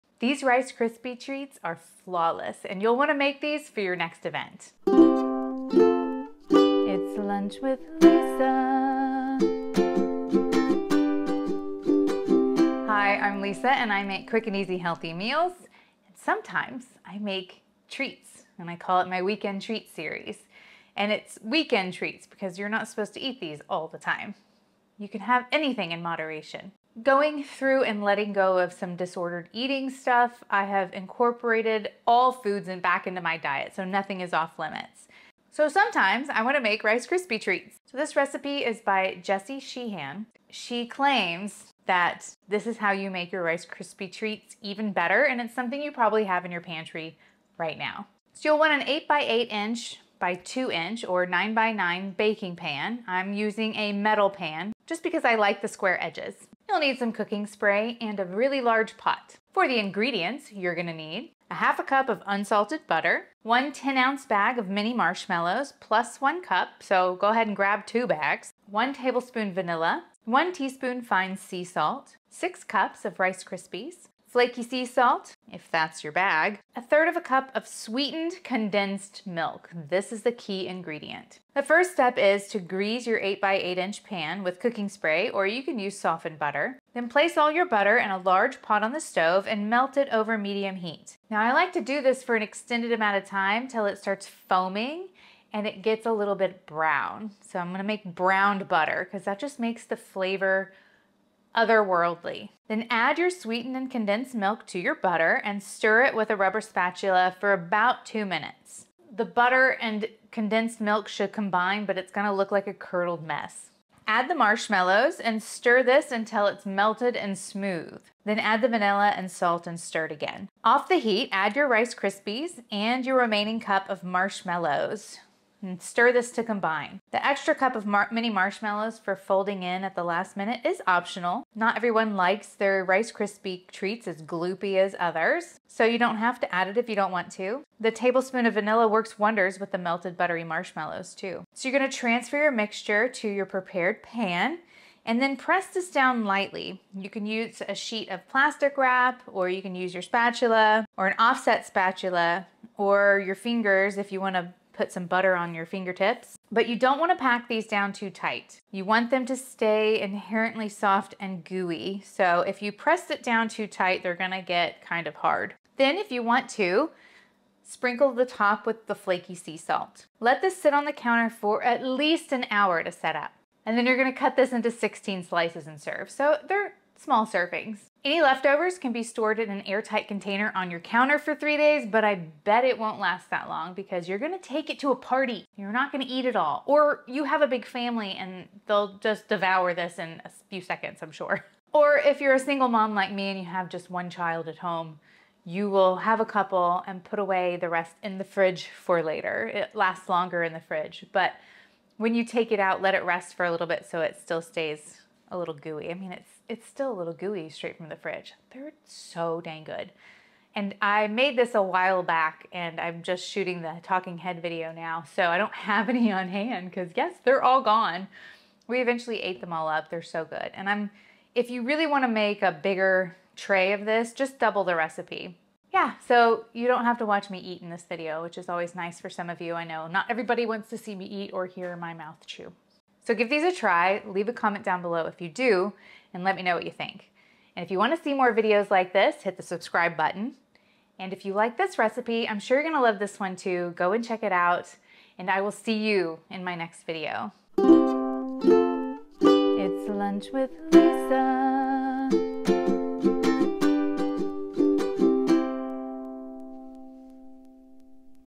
Get Amazing Homemade Rice Krispie Treats with this One Ingredient!
This one ingredient will make your homemade Rice Krispie Treats incredibly soft and chewy! And with a few more tweaks, you won't be able to stop eating these things.
Square baking pan
Oil Sprayer
Large Pot
Offset spatula

Music for intro created and played by me.